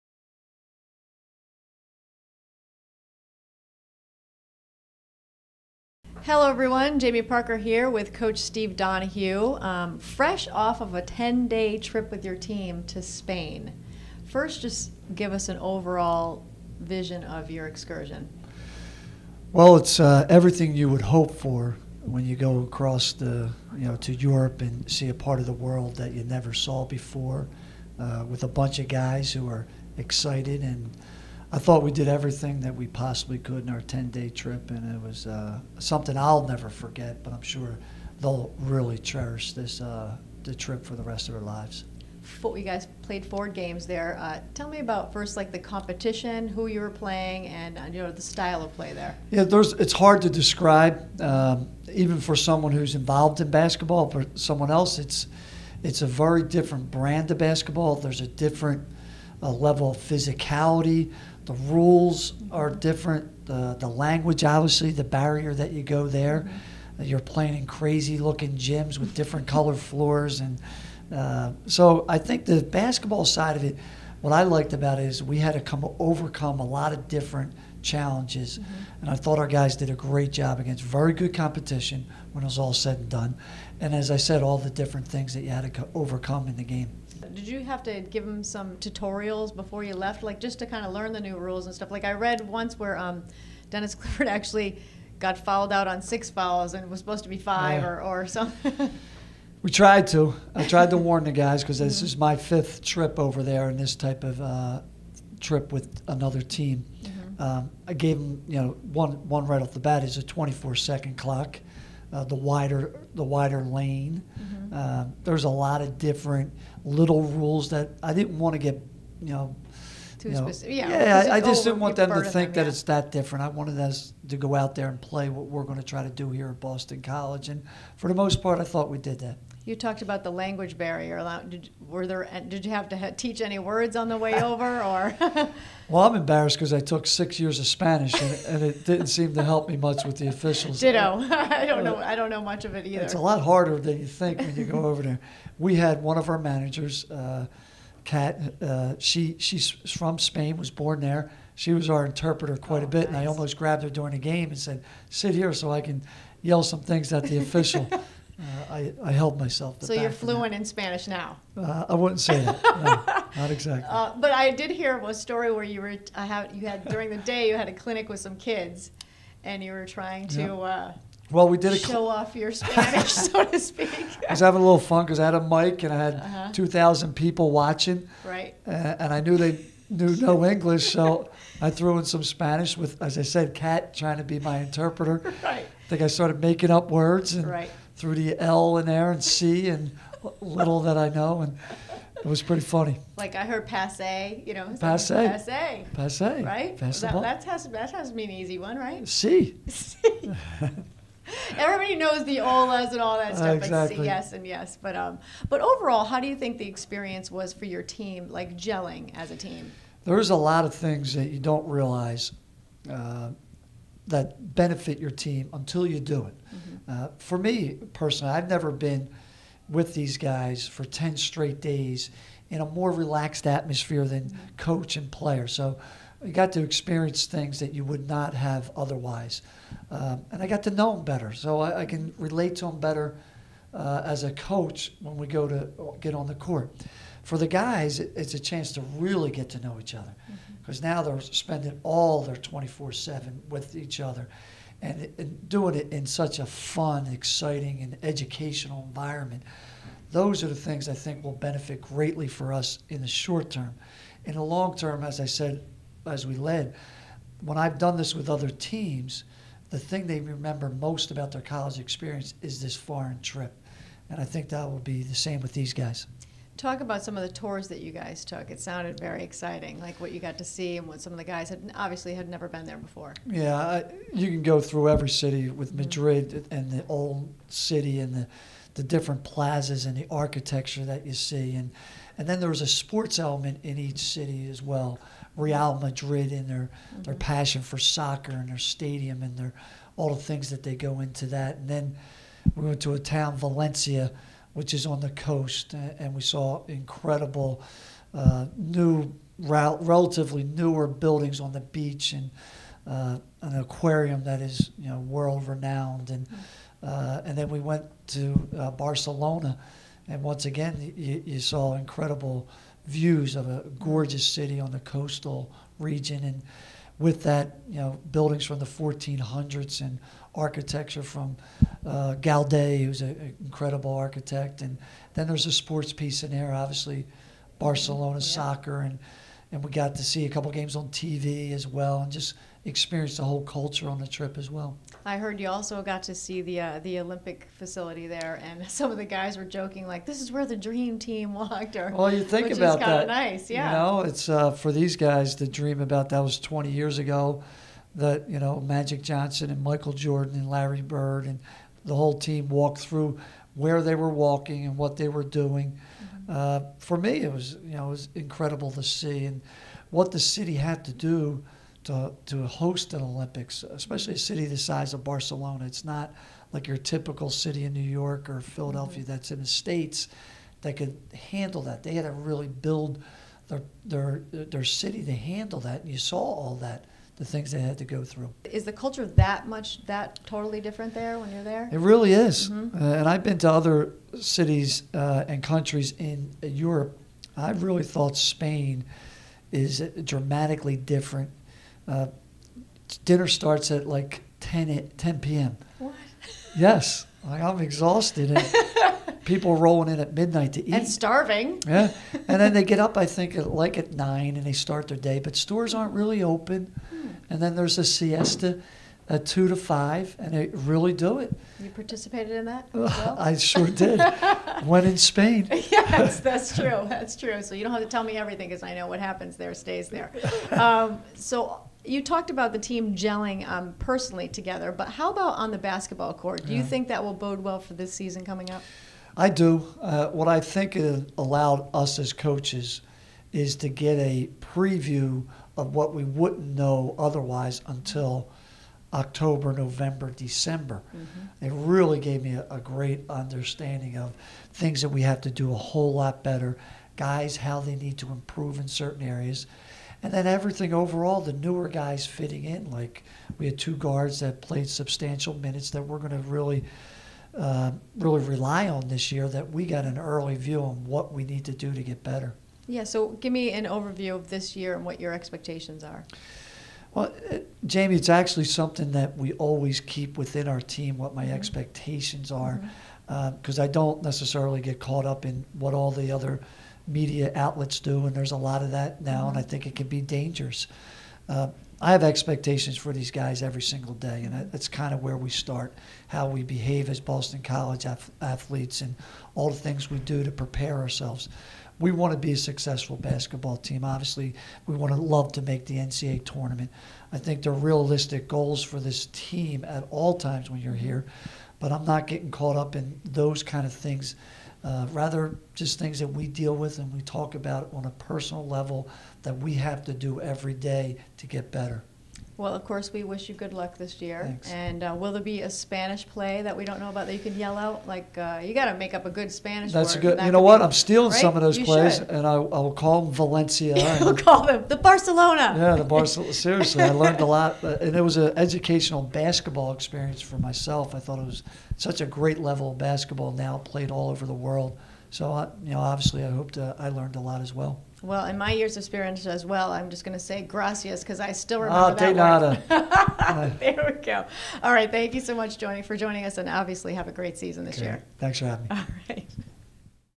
Coach Donahue talks about the team's recent trip to Europe
Coach Donahue talks to Jamie Parker about the Eagles's recent trip to Europe!